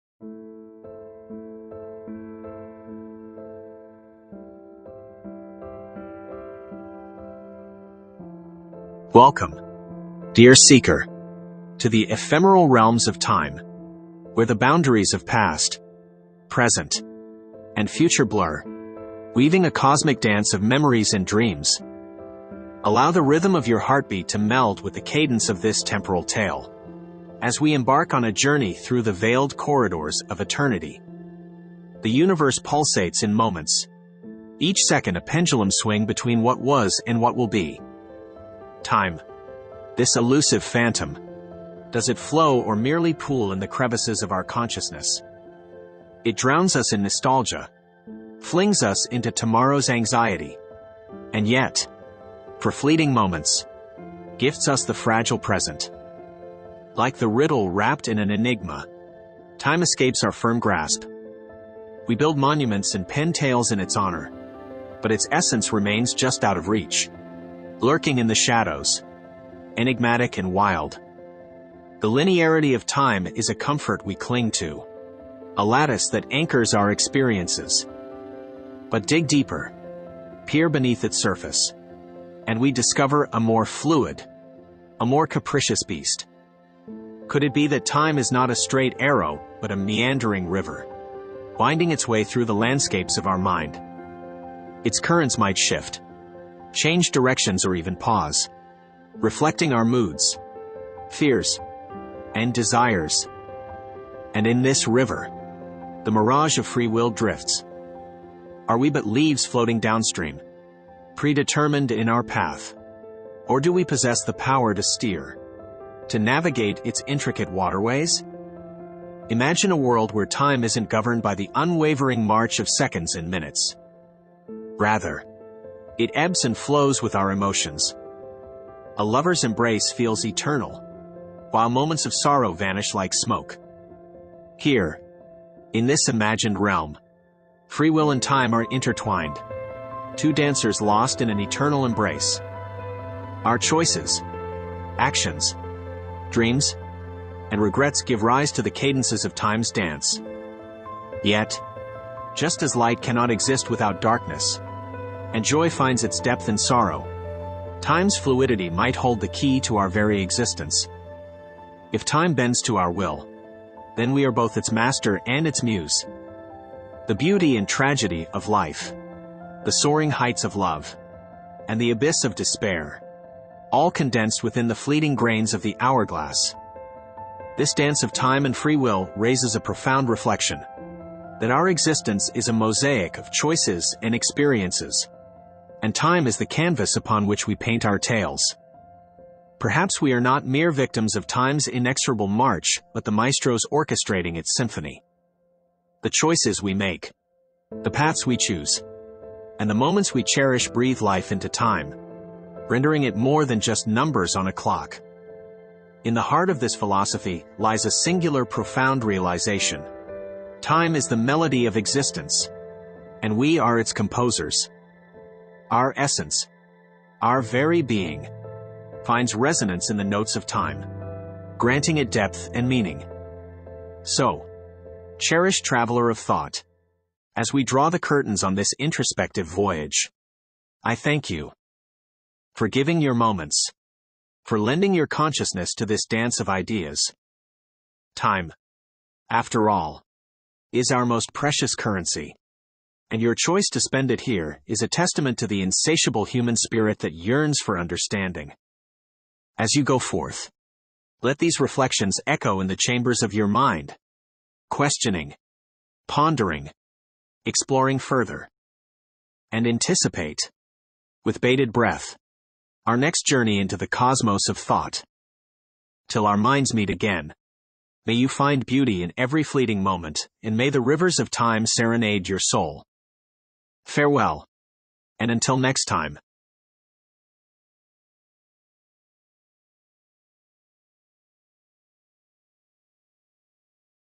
A Dance Between Free Will and Eternity
Dive deep into the enigmatic realms of time where the lines between past, present, and future blend. Is time a straight arrow or a winding river, echoing our emotions and desires? Explore the intricate relationship between free will and time's fluidity. Experience the emotion of moments that feel like an eternity, and ponder on the power of choices in orchestrating the symphony of existence.

Join us on this captivating journey and discover why time might just be the most profound melody of our existence. Every tick and tock holds a universe of thought.